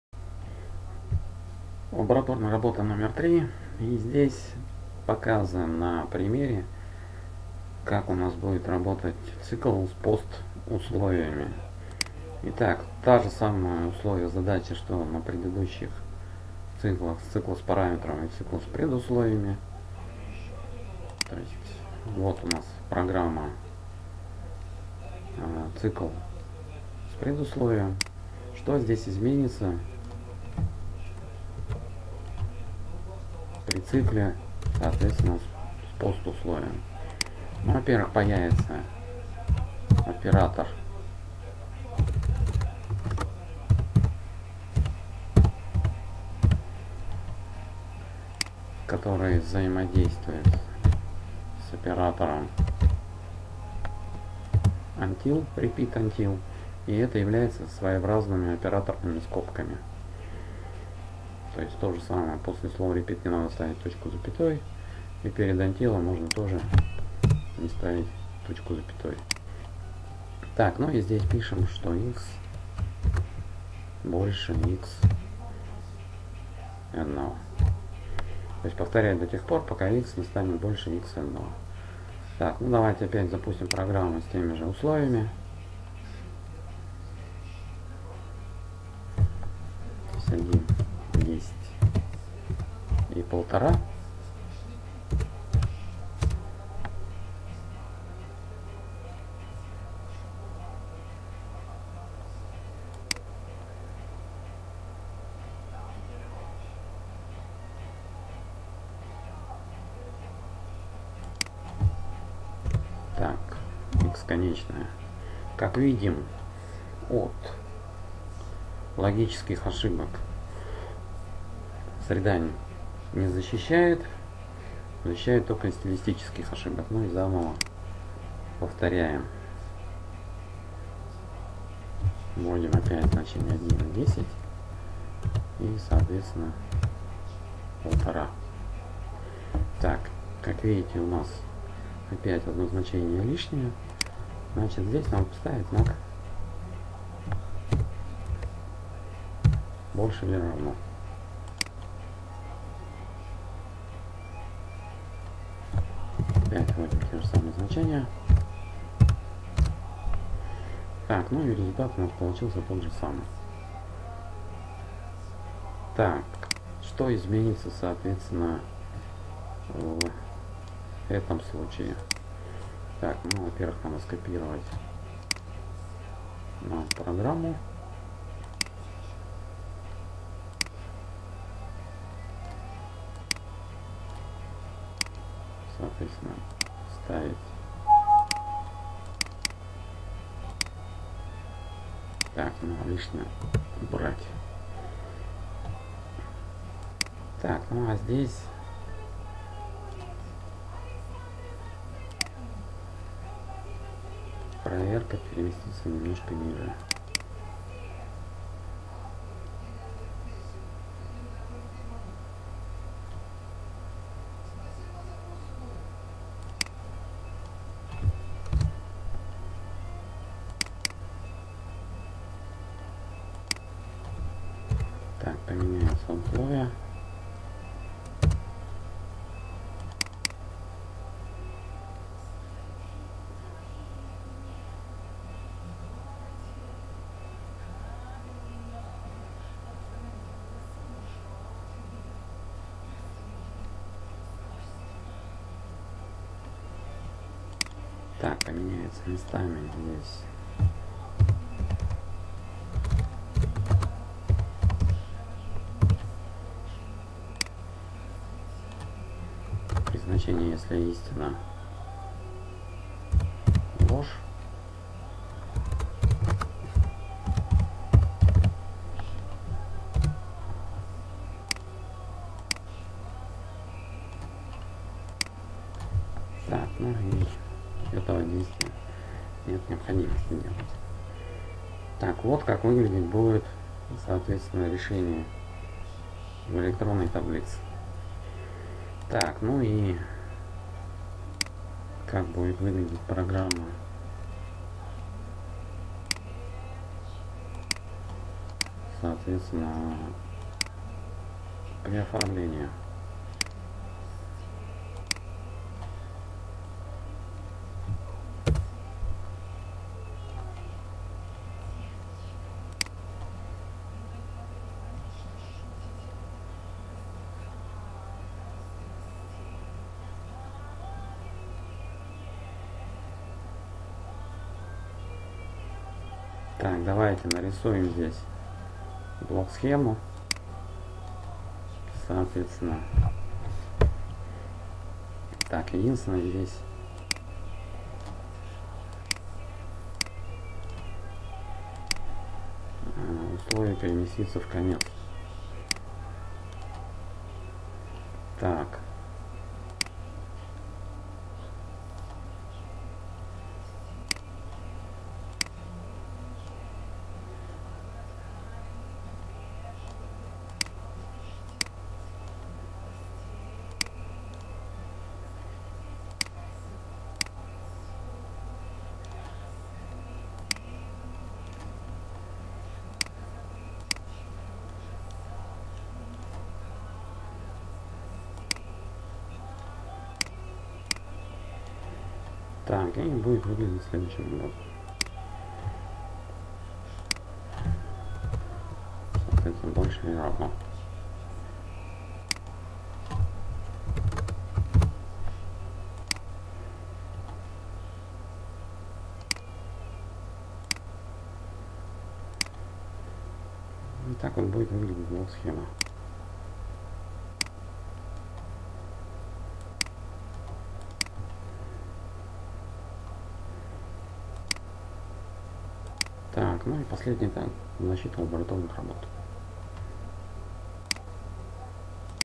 ИНФОРМАТИКА 1. S01.E14. ЛАБОРАТОРНАЯ РАБОТА №3. Пример 5 (Pascal, excel, word)
КОМАНДЫ ПОВТОРЕНИЯ НА ЯЗЫКЕ ПАСКАЛЬ. ЦИКЛ С ПОСТУСЛОВИЕМ UNTIL.
В  ВИДЕО ПРЕДСТАВЛЕН ПРИНЦИП ВЫПОЛНЕНИЯ РАБОТЫ В ЯЗЫКЕ ПРОГРАММИРОВАНИЯ ПАСКАЛЬ, ТЕКСТОВОМ РЕДАКТОРЕ И ЭЛЕКТРОННОЙ ТАБЛИЦЕ ДЛЯ СТУДЕНТОВ НАПРАВЛЕНИЯ "СТРОИТЕЛЬСТВО" ПРОФИЛЬ "ТГВ" И ДР.

0:00 Введение - условие задачи
0:22 Программа задачи на Pascal,
2:47 Электронная таблица задачи excel
4:49 Оформление задачи в текстовом редакторе  word
6:40 Заключение